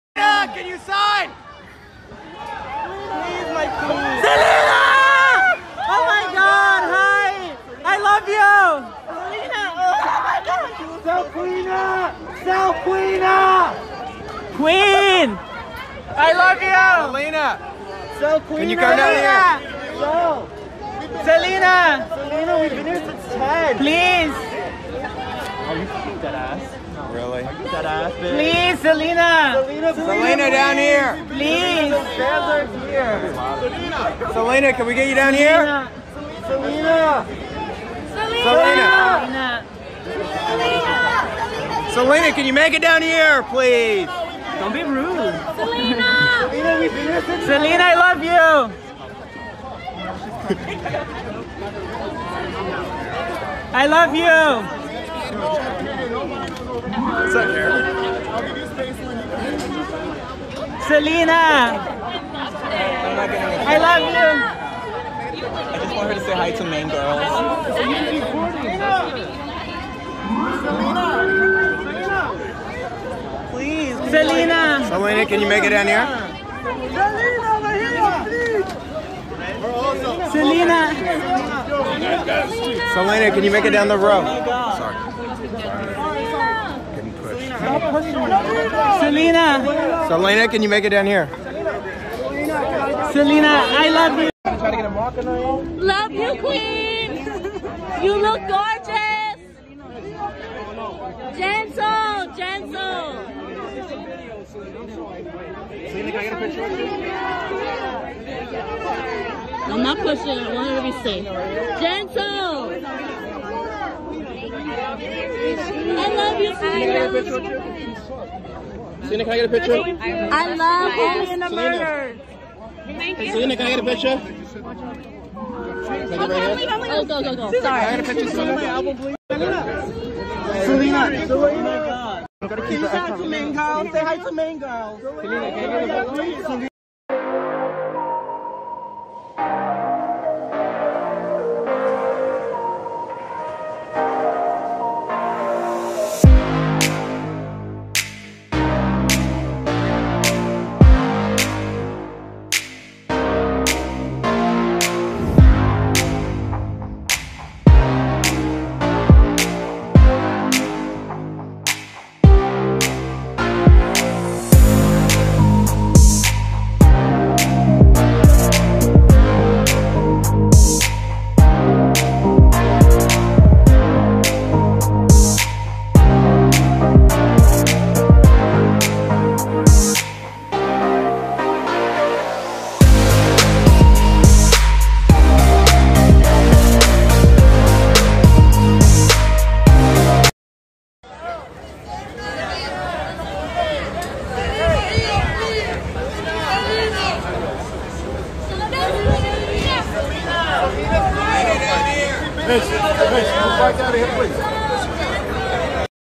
Selena Gomez arriving at "The Late Show With Stephen Colbert" studios in New York City.
Selena Gomez llegando a los estudios de "The Late Show With Stephen Colbert" en ciudad de